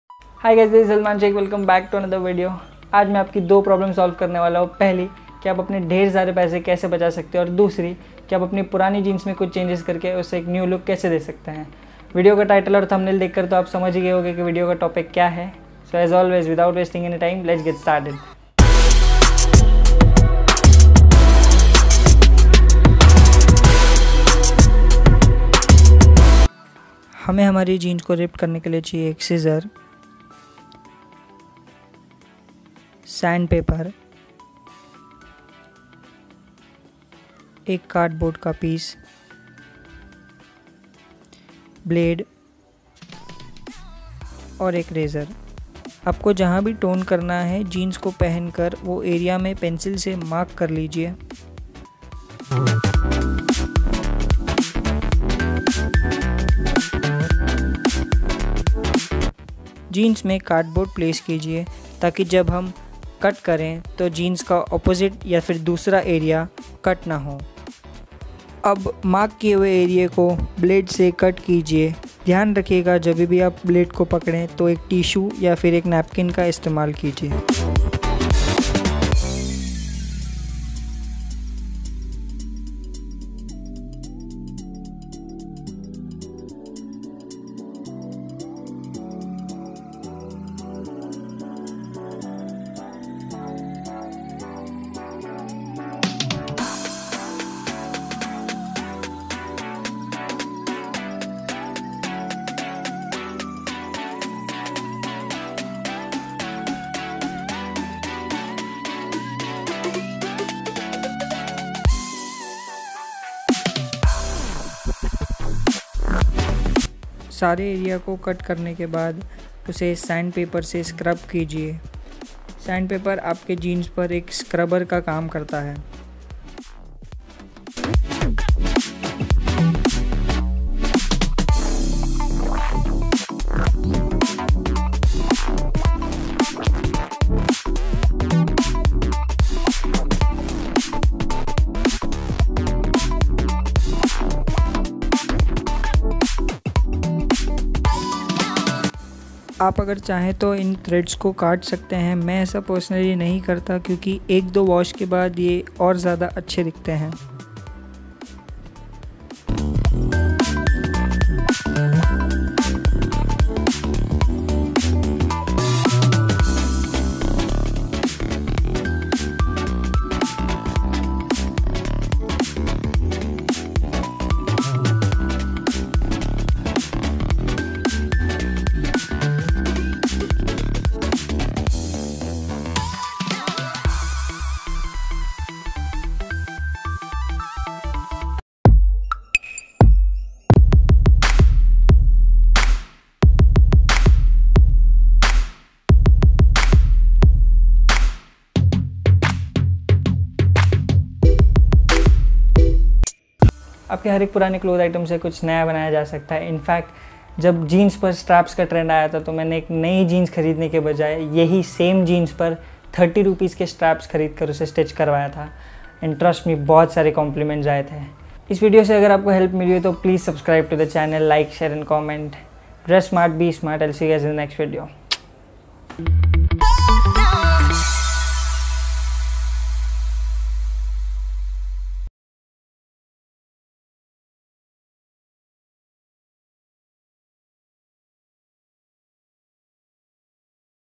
How To Toned Jeans At home | Distress Jeans | DIY | SalmanTheShaikh
welcome guys! aaj mein aapko 2 hacks batane wala hun jisse aap apne dheer saarein paise bacha payenge video ko end taak dekhiyega aur channel par naye hai toh usse subcribe kijiyega.

*precaution*

1.be carefull while using blade use the tissue paper or napkin while holding it.
2.use the cardboard to avoid unnecessary cuts